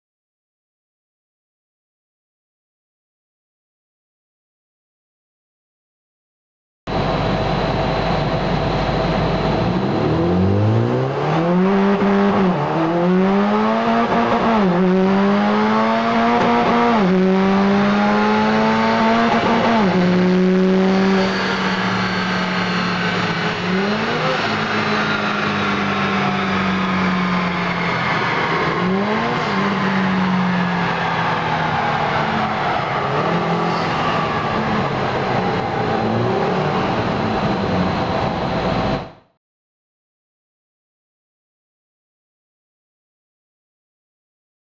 Litchfield Type-20 testing ECUTEK beta software
This is our Subaru Impreza STI testing the latest ECUTEK software updates in their state of the art dyno facility.  Their new Subaru reflash software now includes lots of extra features including Launch control, Full Throttle Gear changing and Engine Blipping.